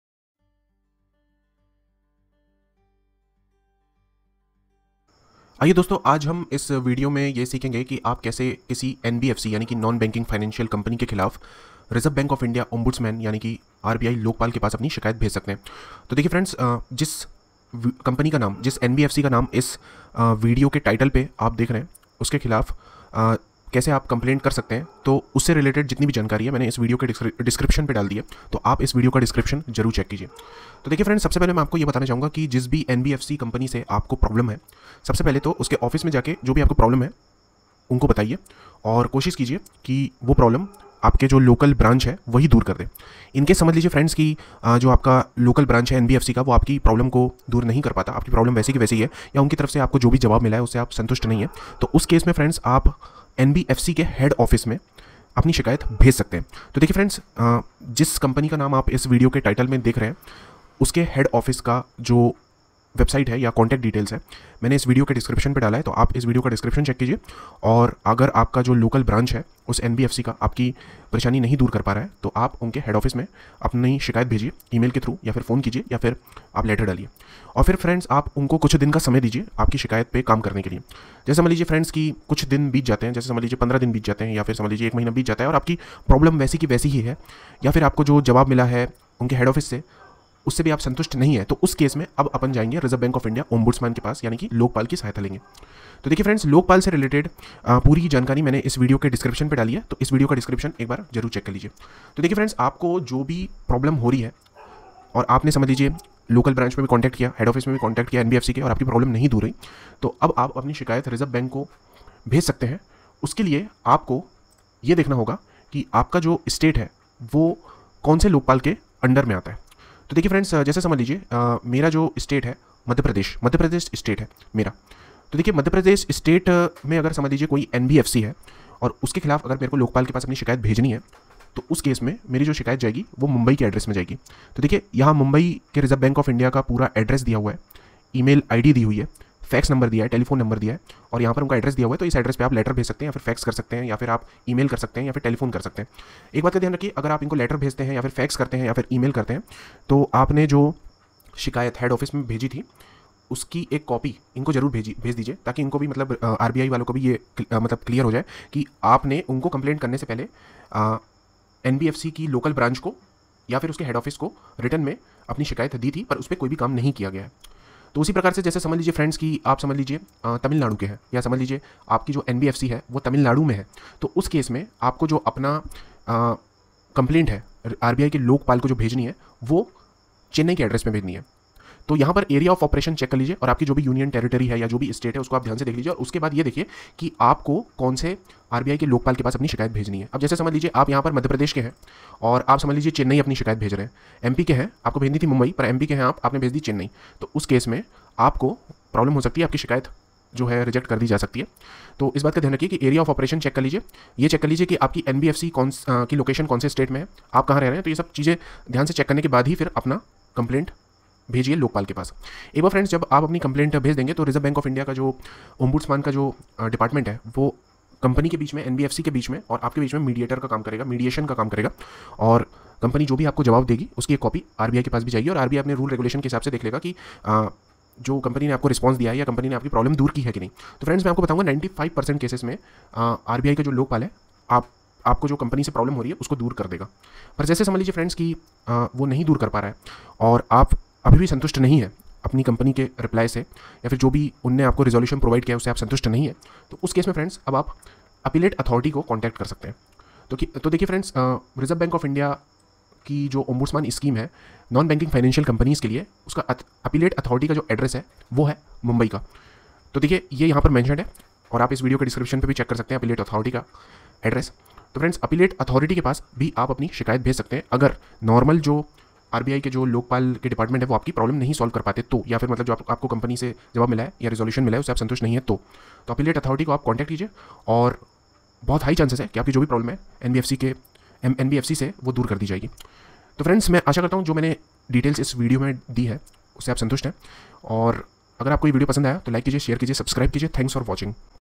Complaint against HDB Financial Services (NBFC) with Reserve Bank of India Ombudsman
In this video we will learn how you can File a complaint against HDB Financial Services (HDBFS is a subsidiary company of HDFC Bank) with Reserve Bank of India under Ombudsman Scheme for NBFC (Non Banking Financial Company)?

While uploading this video there are 4 NBFC Ombudsman of RBI located at Chennai, Kolkata, New Delhi and Mumbai. You should carefully check their area of operation and then only send your grievance.

In case you are not satisfied by the award provided by the NBFC ombudsman (Banking Lokpal) of RBI, then you can further approach the Appellate Authority of RBI. While uploading this video, the address of the appellate authority is:

The Appellate Authority, Ombudsman Scheme for Non-Banking Financial Companies, Consumer Education and Protection Department, Reserve Bank of India, First Floor, Amar Building Fort, Mumbai - 400001.